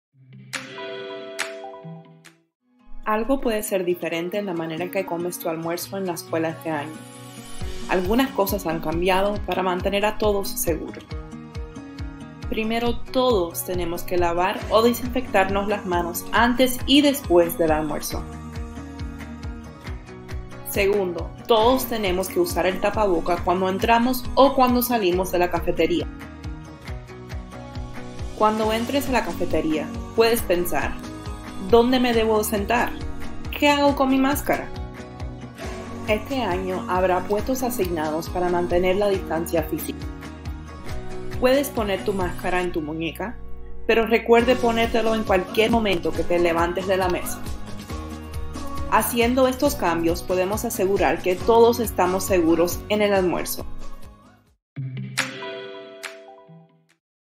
BRST Consejo de seguridad: Como mantener la seguridad en la cafetería
El consejo de hoy va a guiar a estudiantes como mantener la seguridad mientras están en la cafetería